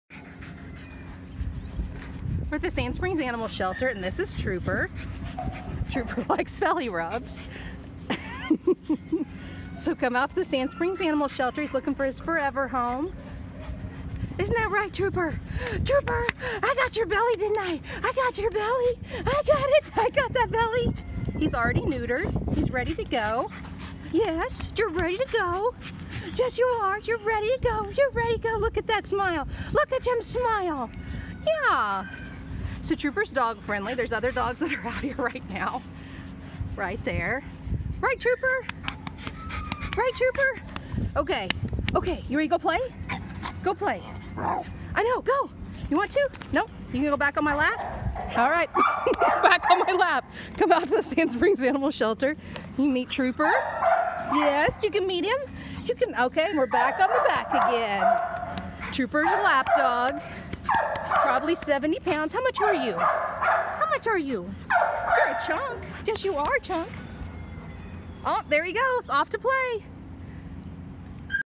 Trooper - Sand Springs Animal Shelter
Trooper - neutered male, dog friendly, very snuggly